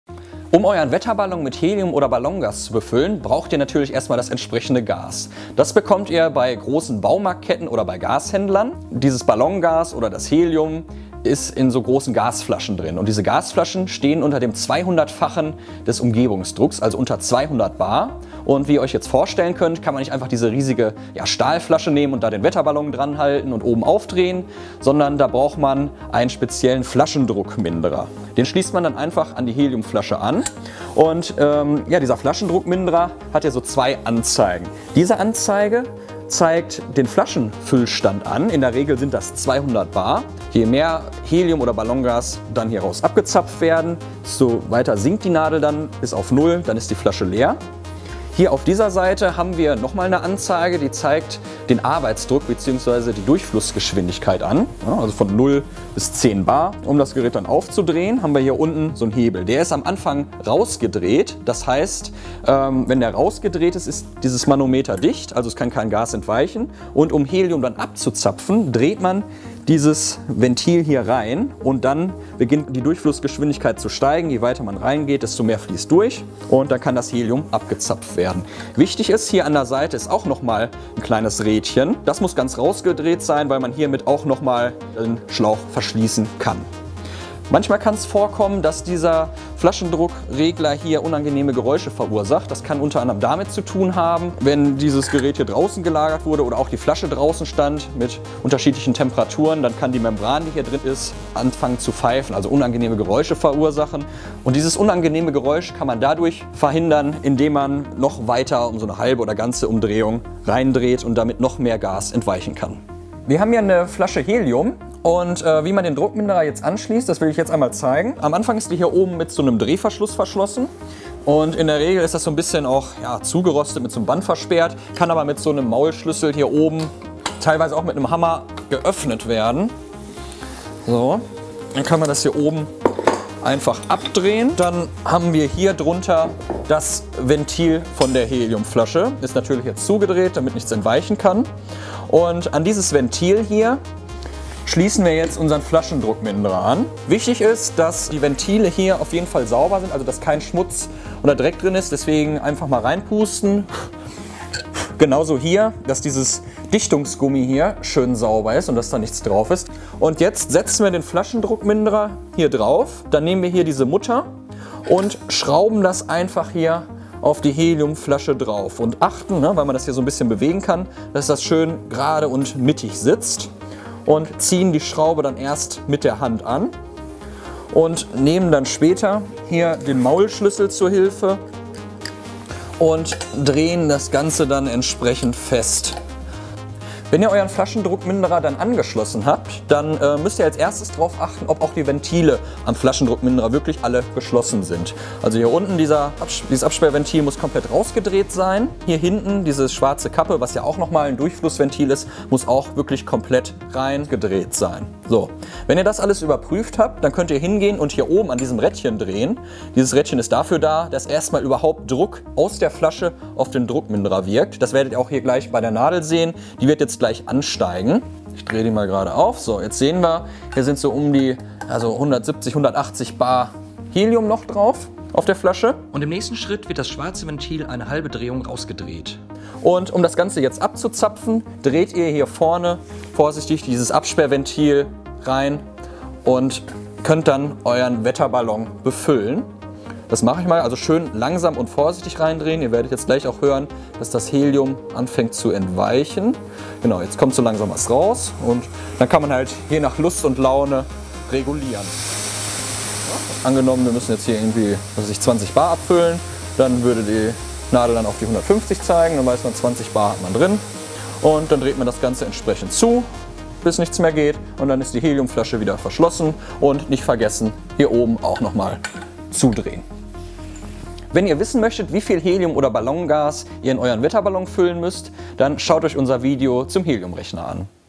Wie funktioniert ein Druckminderer?
Um den Wetterballon mit Helium zu befüllen benötigt man einen Druckminderer. Dafür ist es wichtig zu wissen, wie man diesen anschließt und wie er funktioniert.